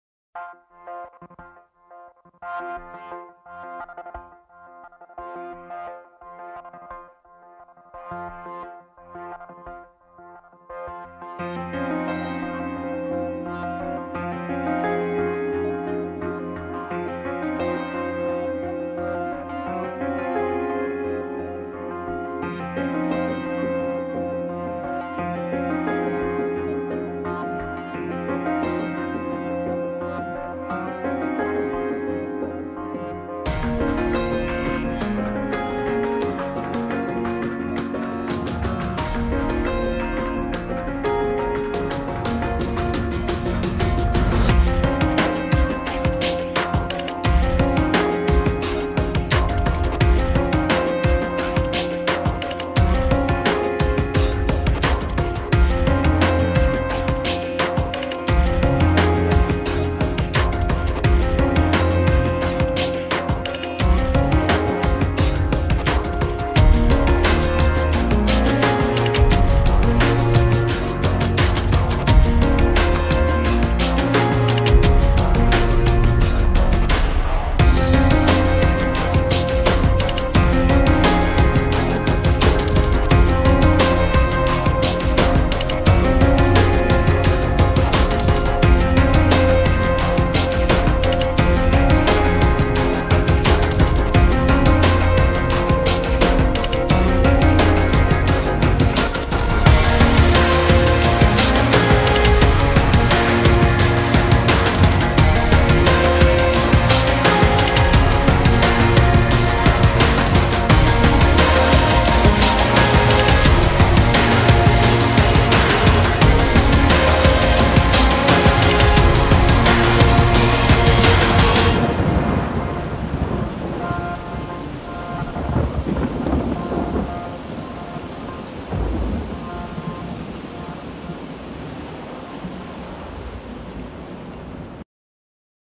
Introduction Part II: Virtual Studio Setup
Including Virtual Instruments, Software Synths and Effects.
A basic default setup for composing and producing music.